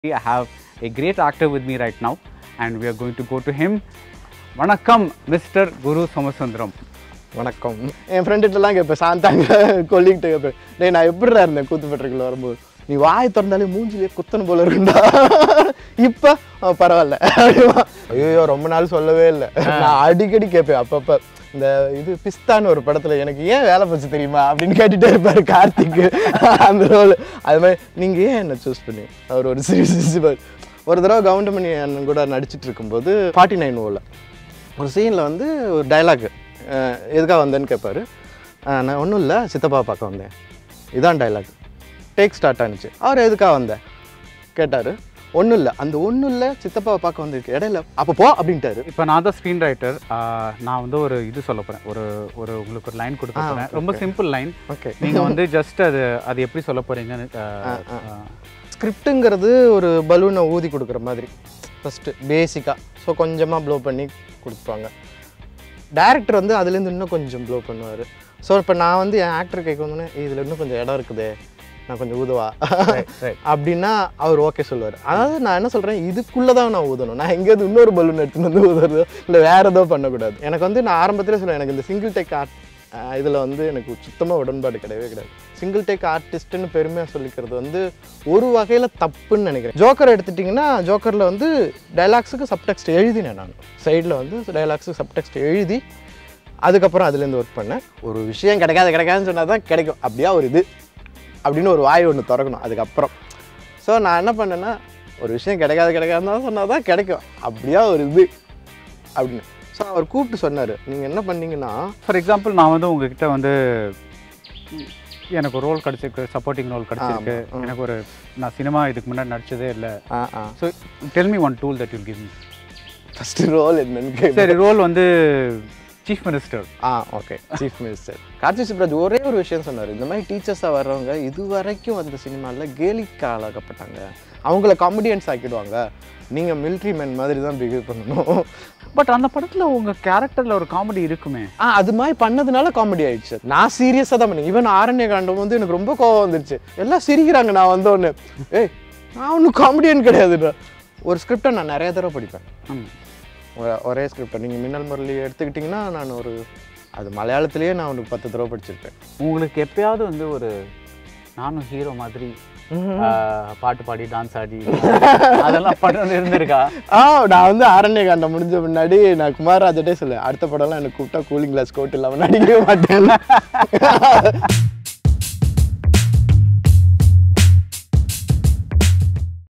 Guru Somasundaram Interview With Baradwaj Rangan | Promo | Masterclass | Minnal Murali | Joker
Actor GuruSomasundaram in this conversation with Baradwaj Rangan talks about how he stepped into acting, how working with ThiagarajanKumararaja in AaranyaKaandam made him a better actor, and why he does not believe in single takes and more!